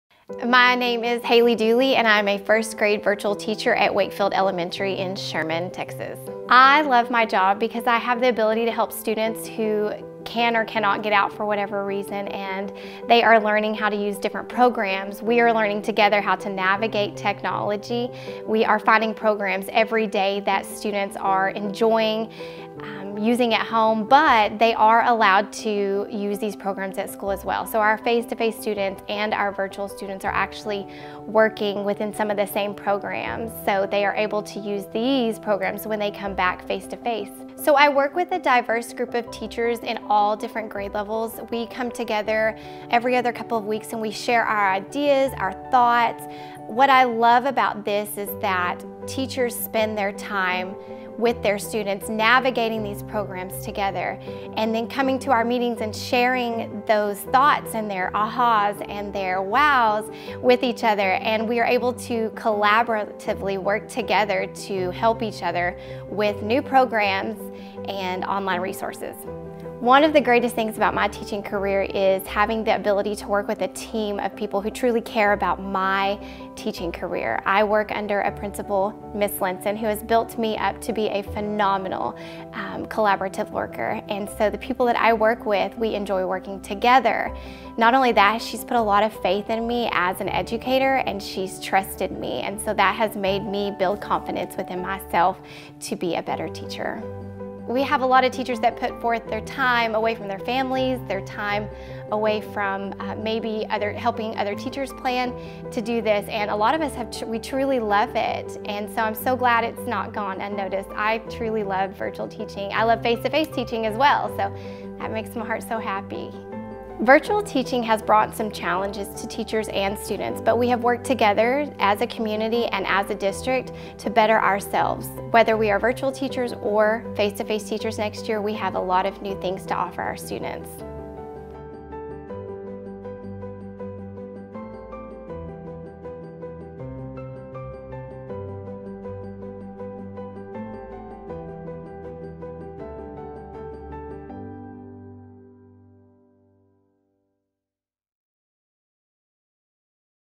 February Elementary Teacher of the Month: Haylea Dooley, Wakefield Elementary
This year, Mrs. Dooley has taken on the lead role to make sure that virtual teachers have an opportunity to communicate as a team. Throughout this unique year, she has planned lessons, created extra tutoring sessions for students and parents, and she has supported face-to-face teachers on campus. She has also served on the Virtual Exploratory Committee to help support our distance learning program. Mrs. Dooley’s virtual teaching efforts were featured on news 12 earlier this year to highlight how Sherman ISD teachers have gone above and beyond to ensure all students remained connected and engaged during virtual learning. Congratulations Mrs. Dooley for being named the February Elementary Teacher of the Month.